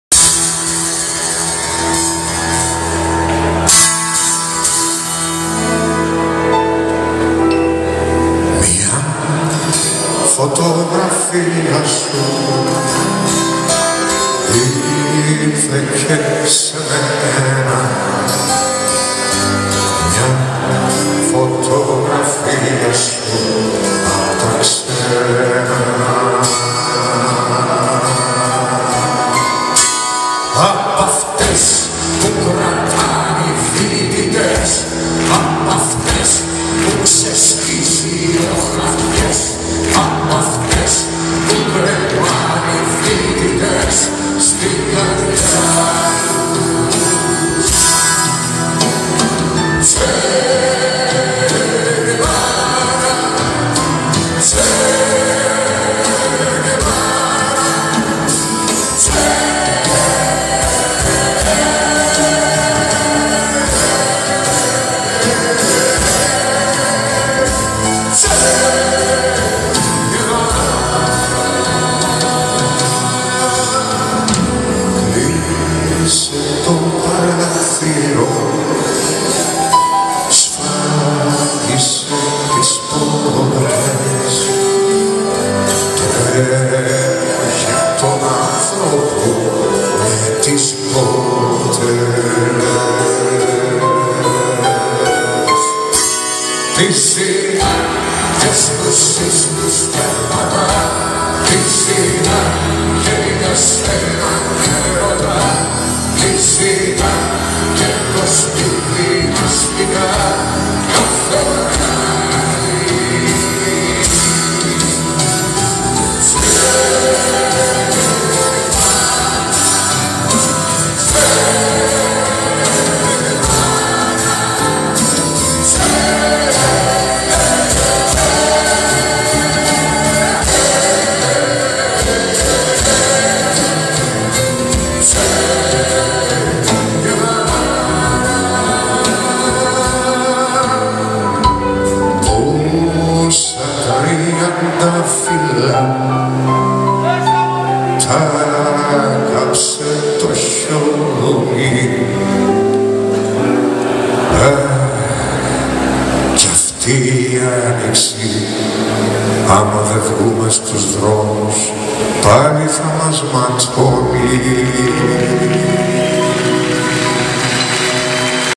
ΒΑΣΙΛΗΣ ΠΑΠΑΚΩΝΣΤΑΝΤΙΝΟΥ-ΙΒΑΝΩΦΕΙΟ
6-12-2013 Συναυλία του Βασίλη Παπακωνσταντίνου στο Ιβανώφειο Στάδιο στη Θεσσαλονίκη.